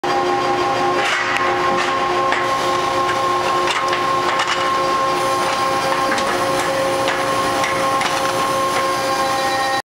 Hydraulic Press | Automated 350-ton | French Oil Mill Machinery Co.
A French automated 350-ton hydraulic press runs through the mold service cycle.  This sideplate press is part of an 8-press system custom designed for a customer molding seals for the oil and gas industry.